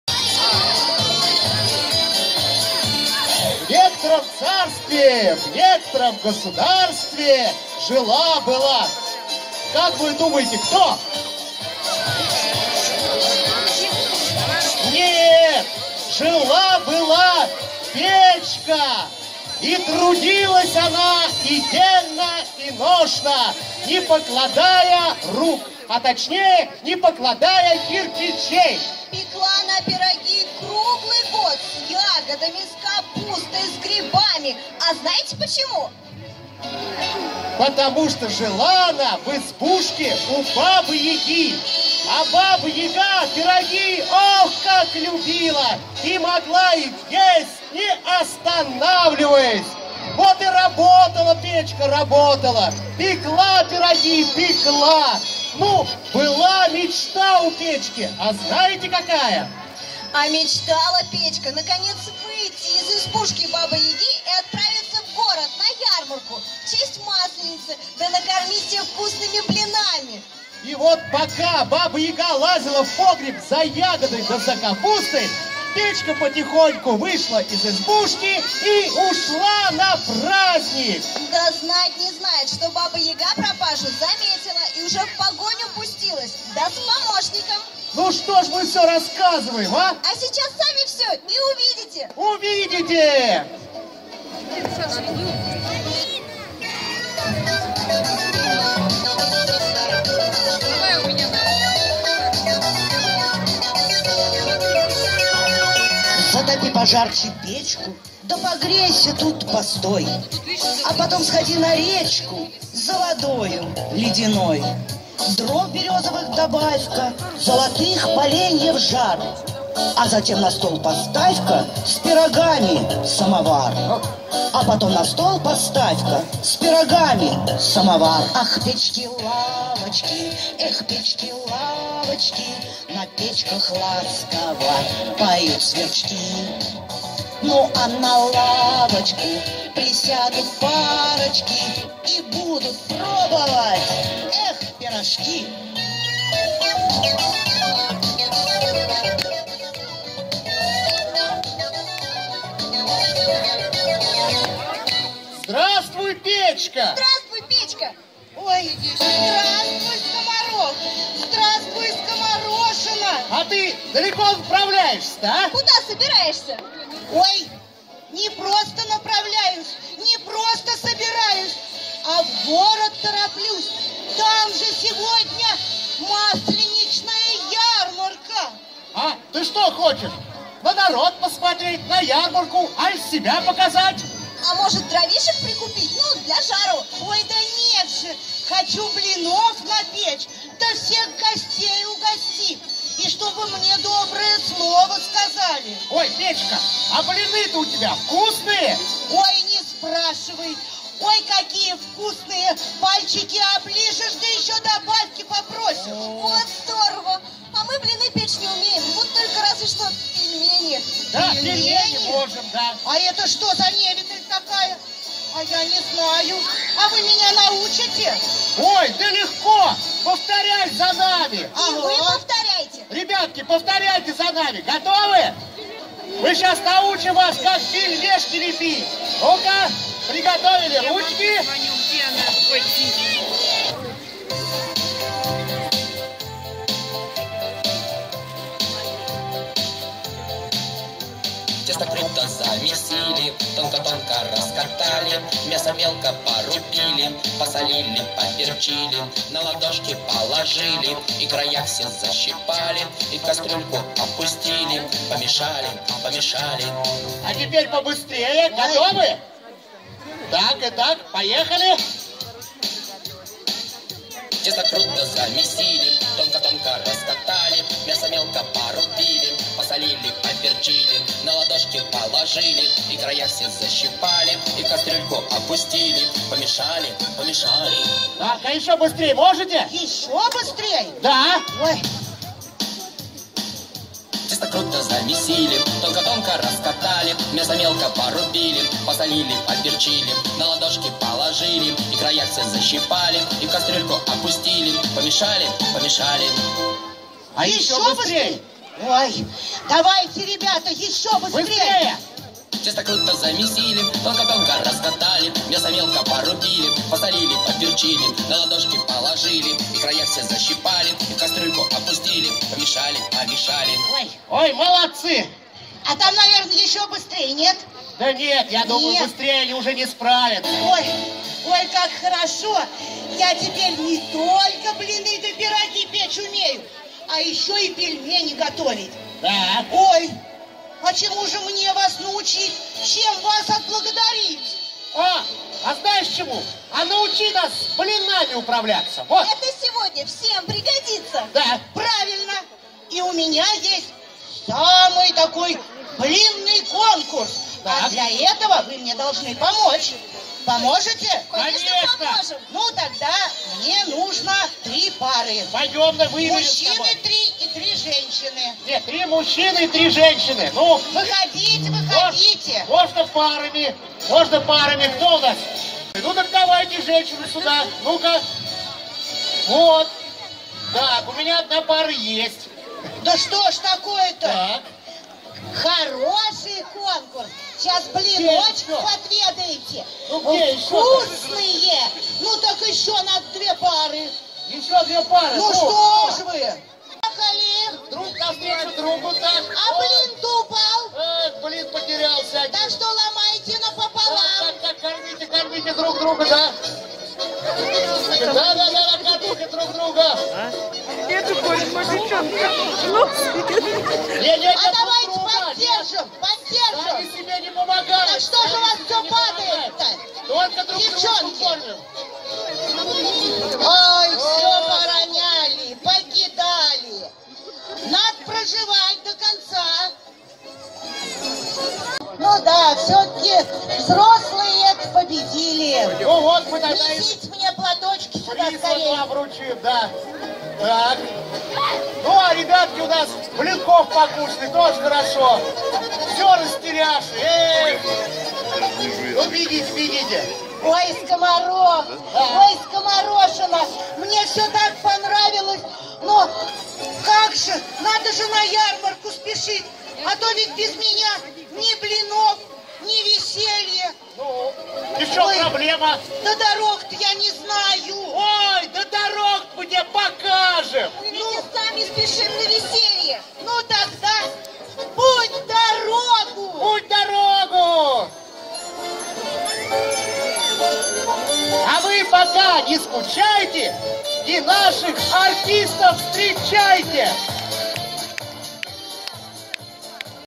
Сказка Жила Была Печка / Fairy tale Was there a Stove
Масленица в микрорайоне Авиационный города Домодедово

Заработок без вложений и риска - e-Commerce Partners Network
Присоединяйтесь к партнерской программе e-Commerce Partners Network!